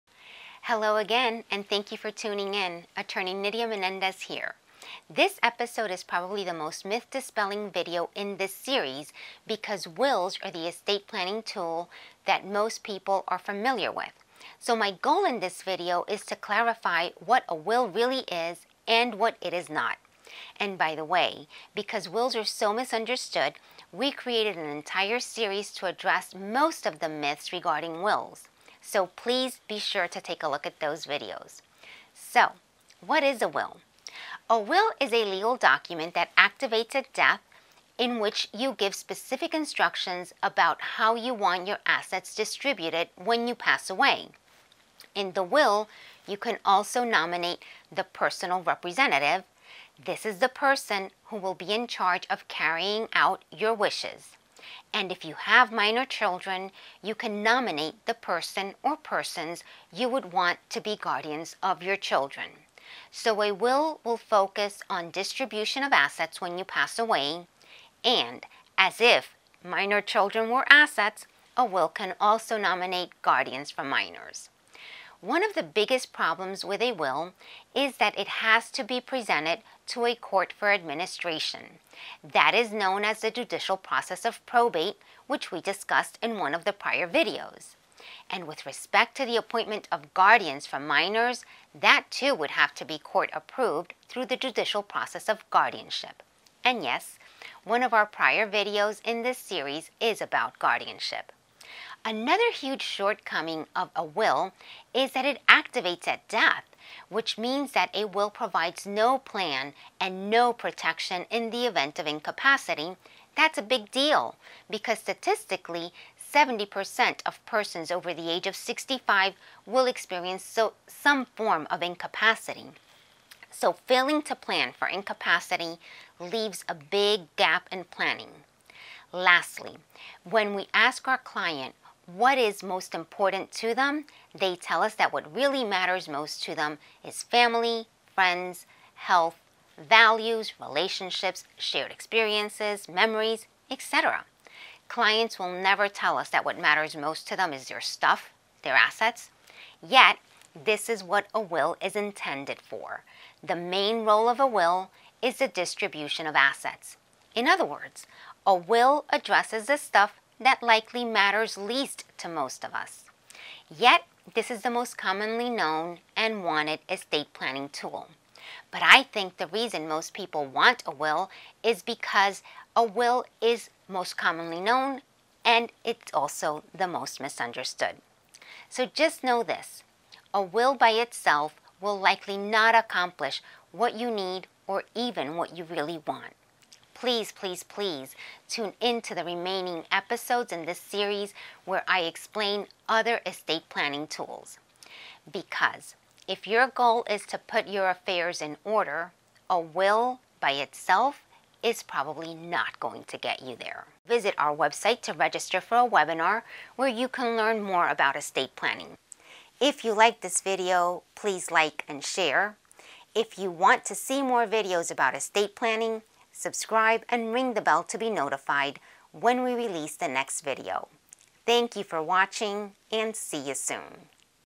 Estate Planning 101 - Episode 4: What is a Will?
This episode is probably the most myth-dispelling video in this series because Wills are the estate planning tool that most people are familiar with.